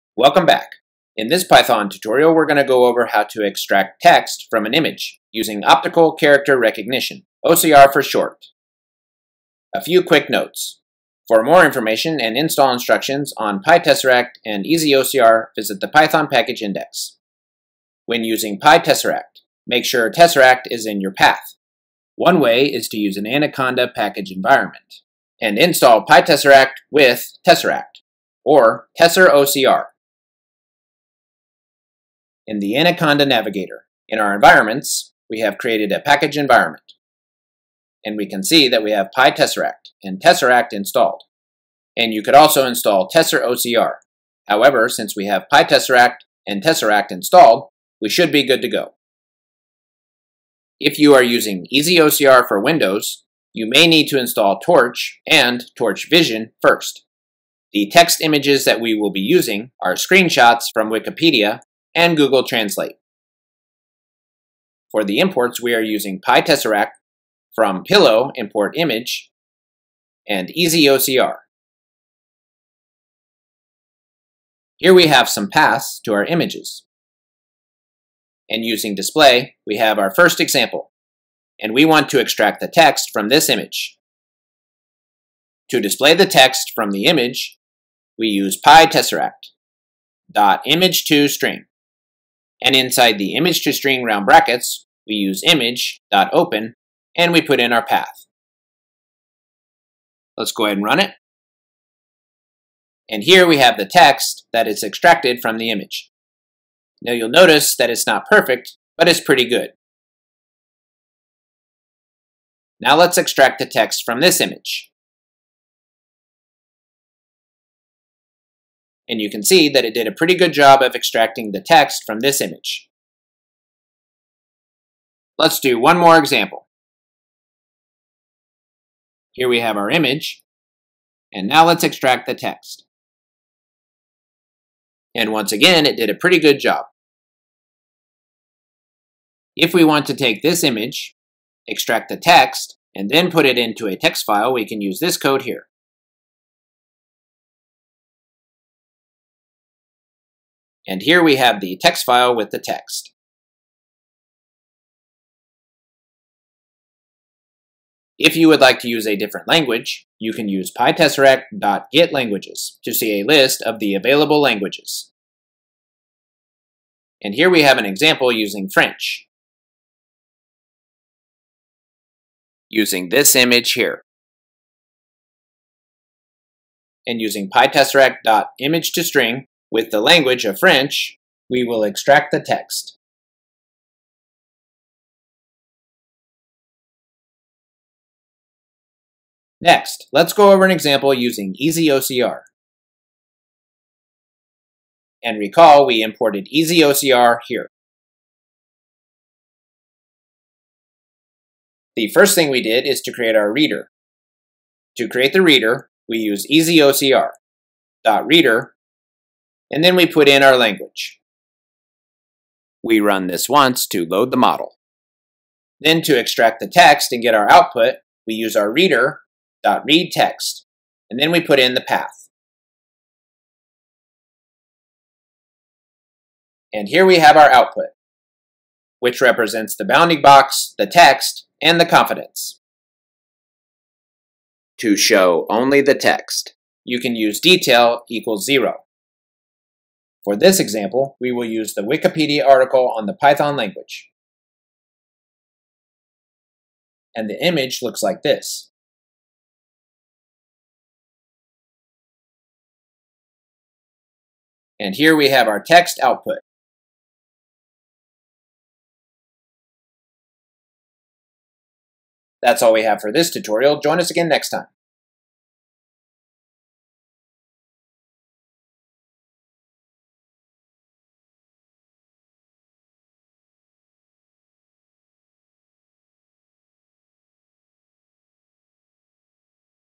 Python Extract Text from Image using OCR
In this python tutorial, we will go over how to extract text (using optical character recognition - OCR) from an image using pytesseract (with tesseract) and easyocr, how to extract text in different languages, and how to create a text file with the extracted text.

Note: If you have trouble installing easyocr, try creating a new package environment with a different python version such as 3.8. When I tried to install using python 3.10 I received an error - I am not exactly sure why and I am guessing this will be fixed eventually. I created a new Anaconda package environment using python 3.8, pip installed easyocr using terminal in that environment and everything worked.

Another Note: I created a new Anaconda package environment for ocr using Anaconda Navigator however the Jupyter Notebook icon was not visible to install so I pip installed it in the new environment using terminal (command line) instead.

install:
pip install notebook

run:
jupyter notebook